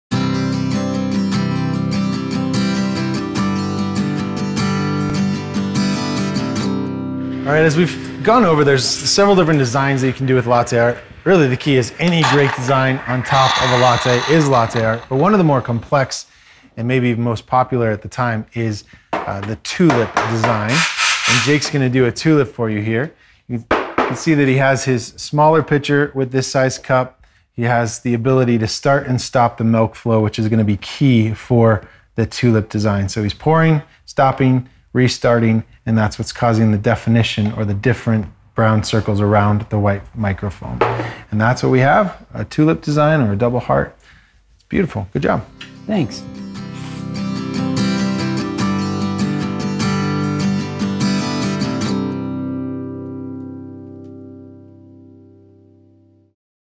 How to Pour A Latte Art Tulip
In this video, Jake Weidmann, artist, and barista, shows us how to pour a tulip in latte art. This is part of the course, Latte Art Pro, found free on our youtube channel.